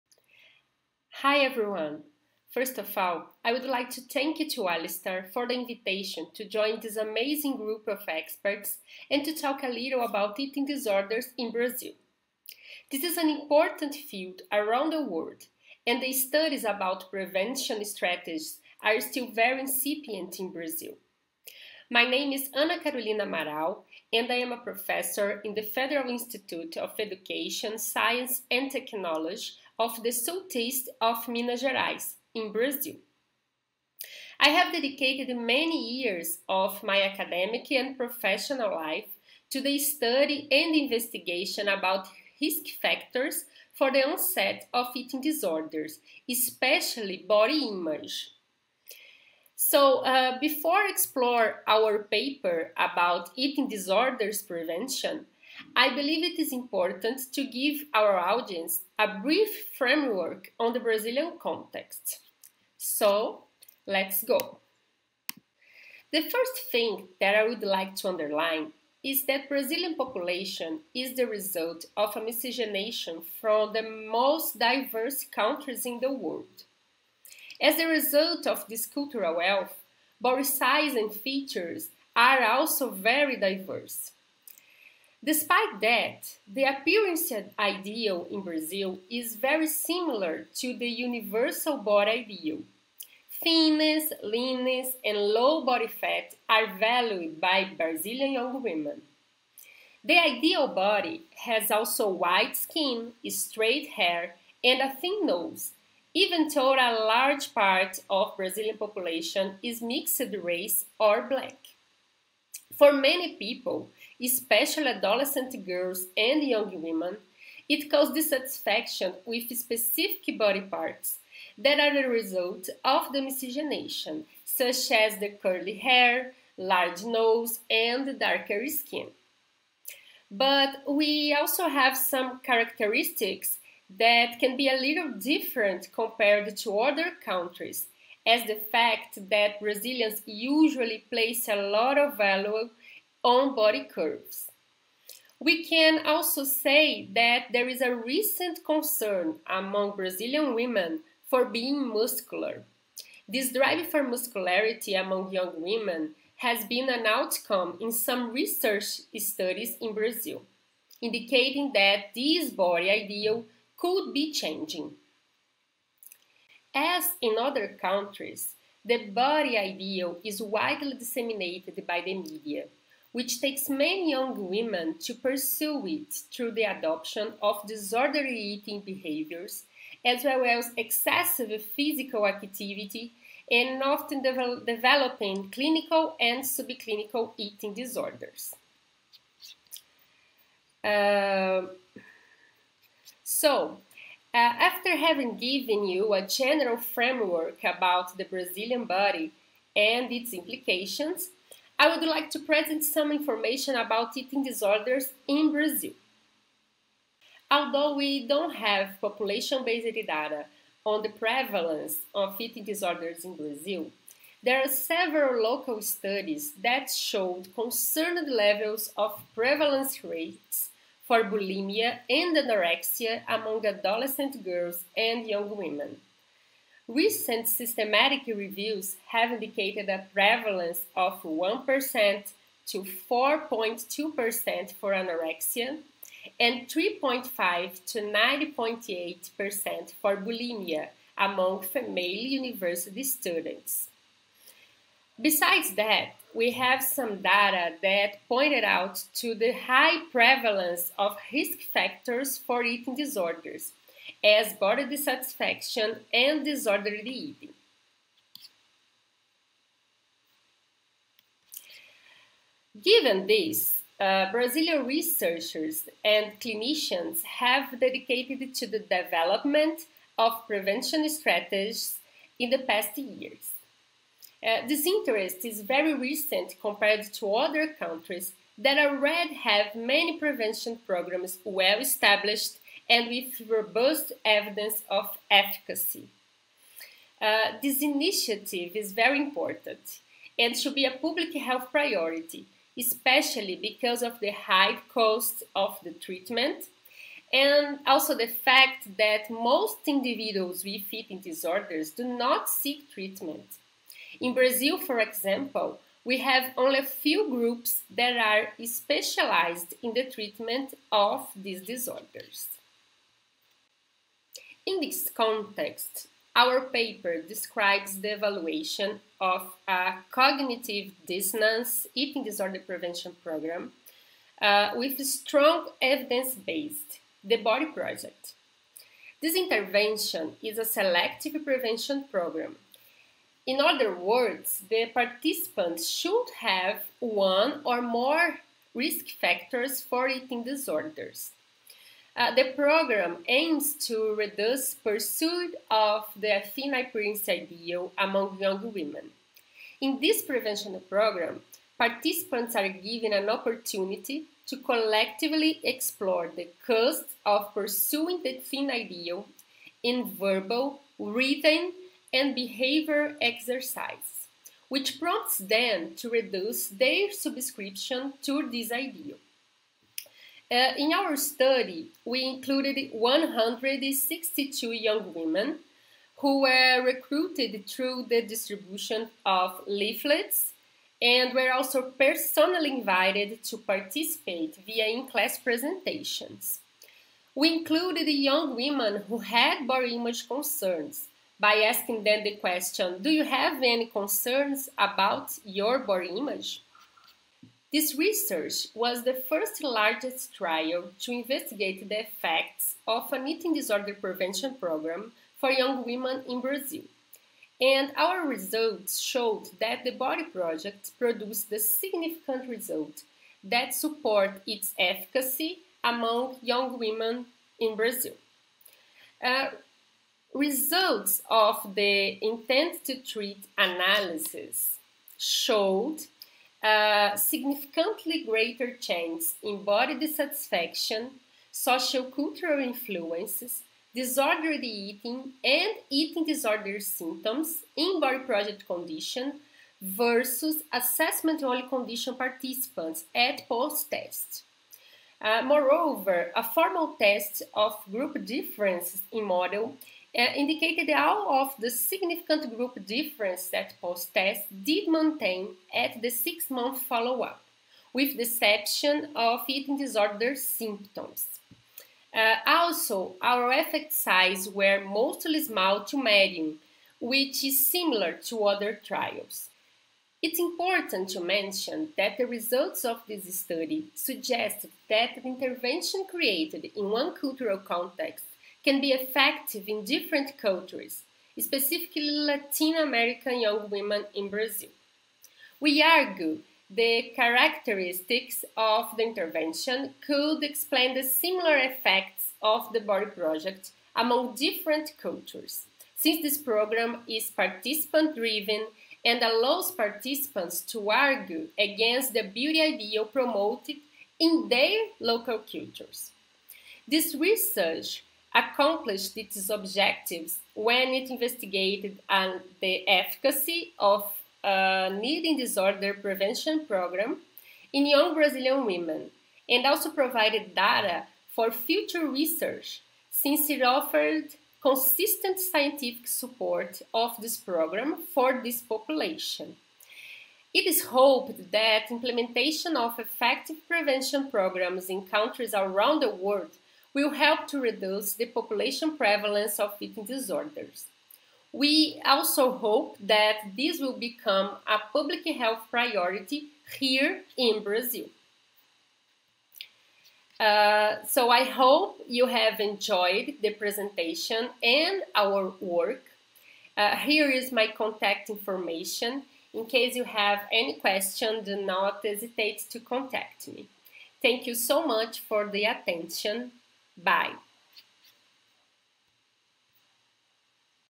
Dr. Ana Carolina Soares Amaral on eating disorder prevention among Brazilian females
Dr. Ana Carolina Soares Amaral speaks about her most recent research on eating disorder prevention among Brazilian females.